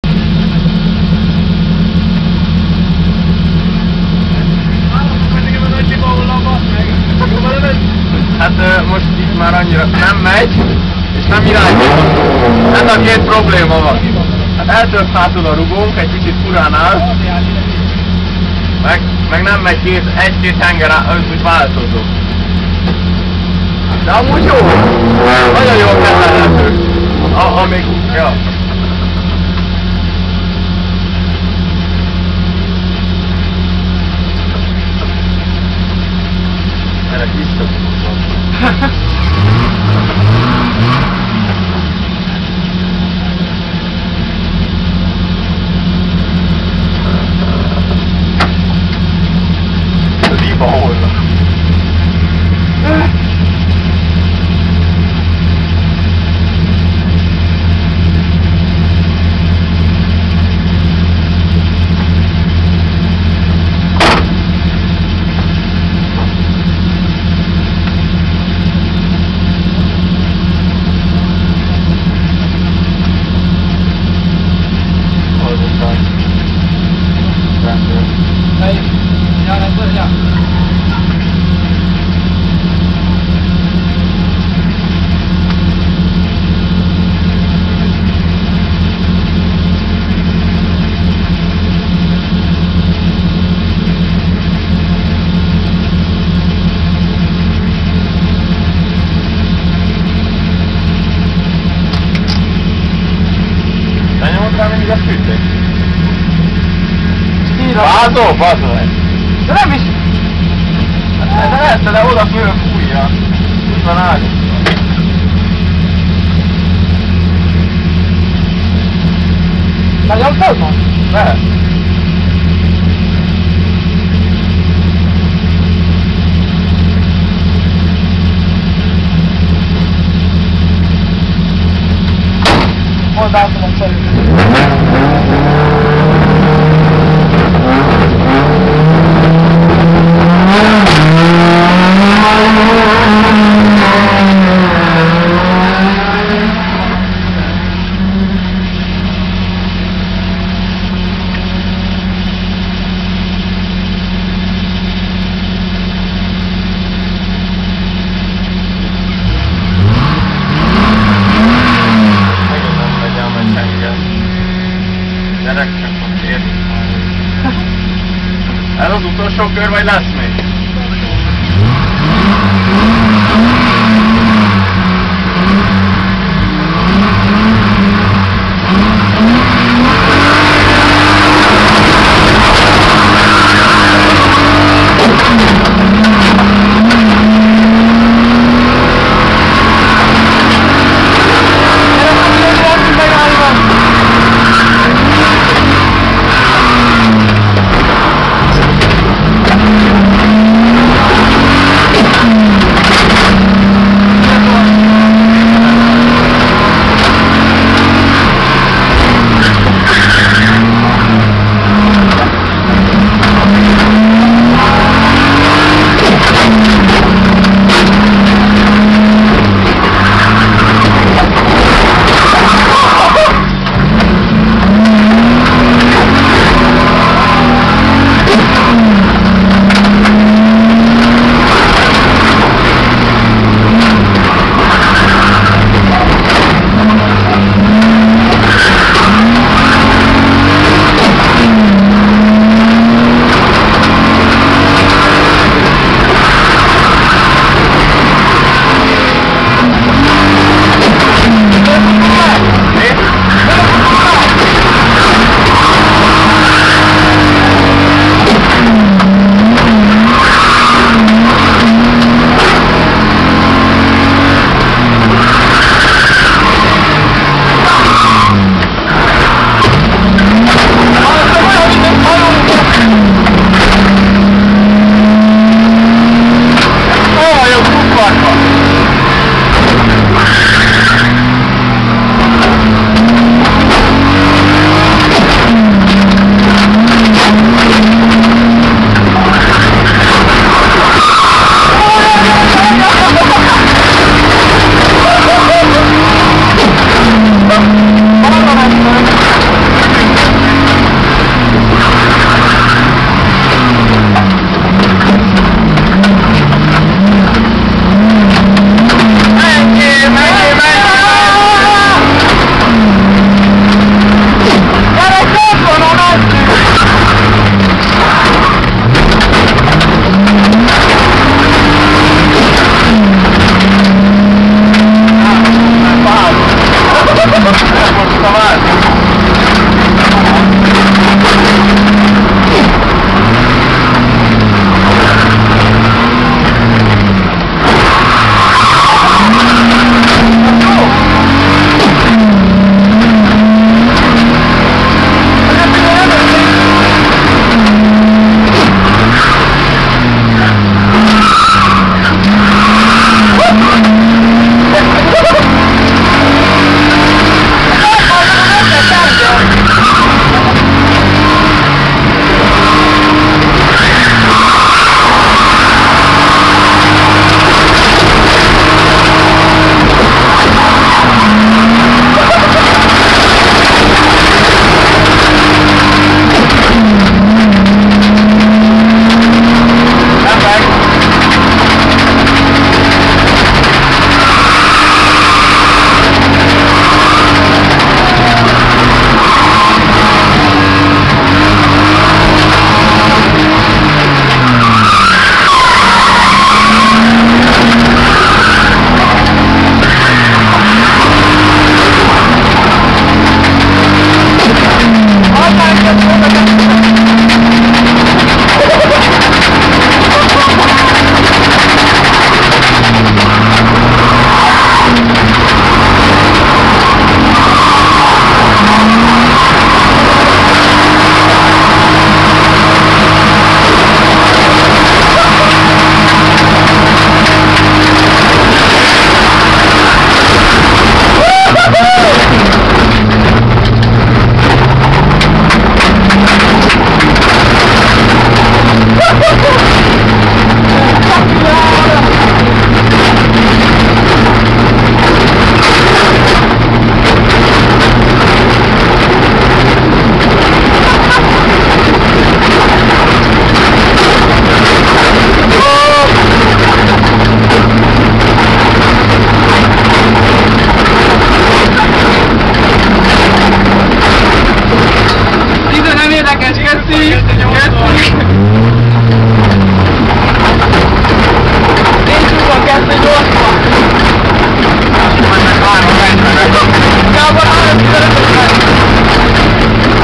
Sándor-Plávics BMW E28
Utolsó kör:

-jobb hátsó rugó elfogyott
-jobb hátsó ablak elveszett :)
-kormányközép soha nem is volt
-a motorból 2 hengert megevett a tehén